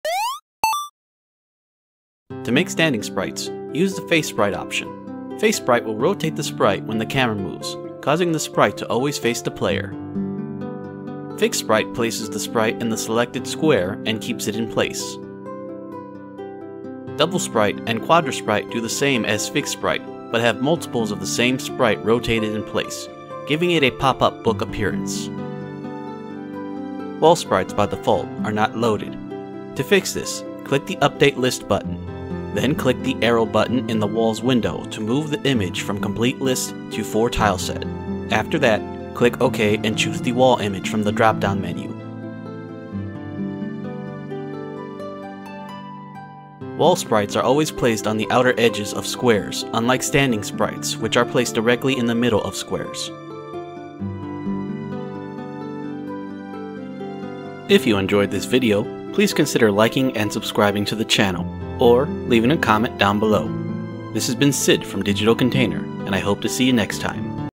RPG Paper Maker Tutorial - #004 Using Sprites
Videos helping newcomers with the game making engine, RPG Paper Maker. This video focuses on the basics of the Map Editor.